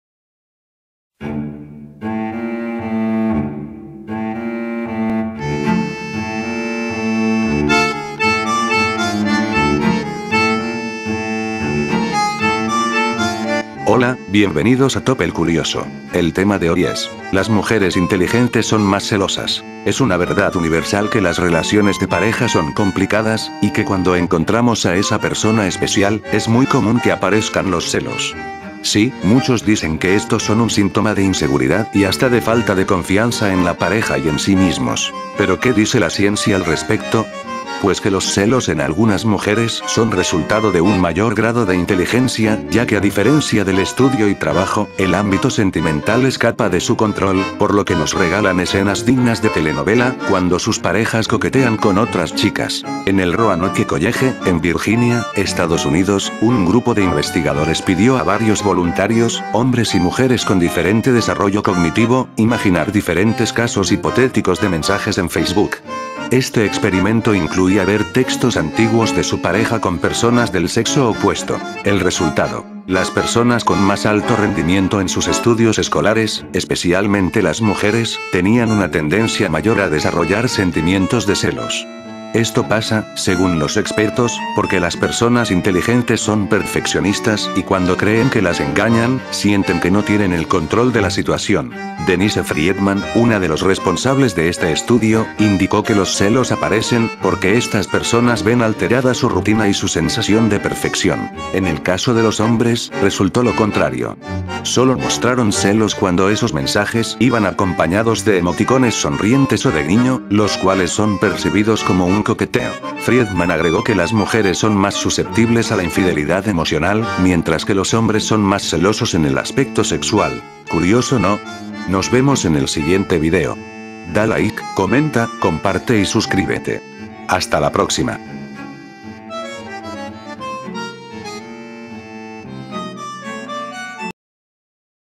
¡Comprobado! Las mujeres inteligentes son más celosas
Muchas personas creen que los celos están relacionados a la inseguridad y baja autoestima, pero no siempre es así